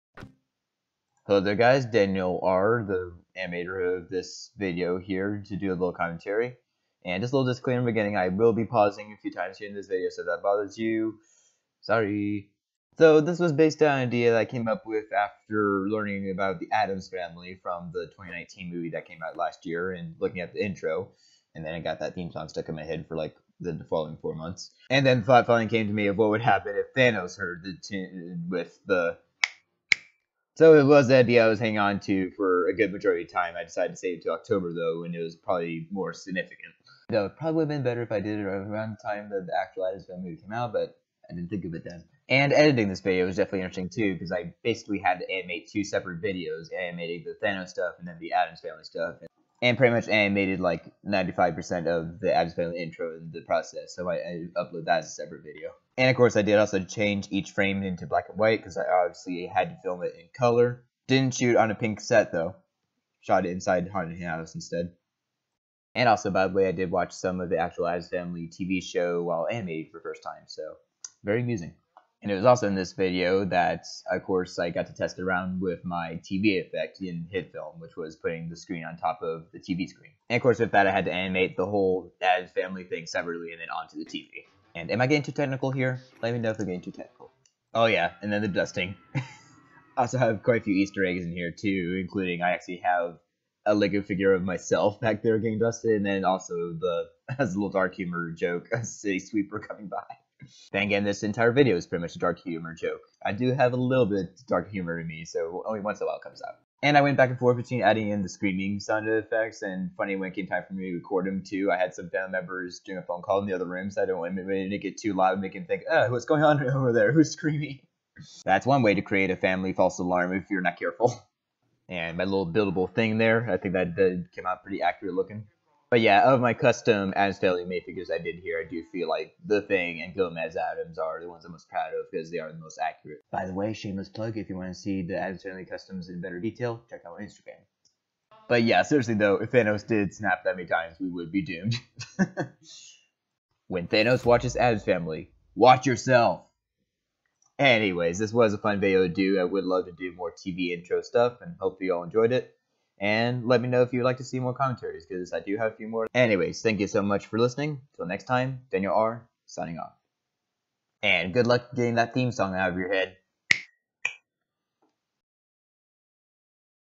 Thanos' favorite TV show - Audio commentary!
It's been a while since I've uploaded an audio commentary, so here's one for my latest stop motion "Thanos' Favorite TV show." Hope you all enjoy and let me know if you would like to hear more commentaries!;)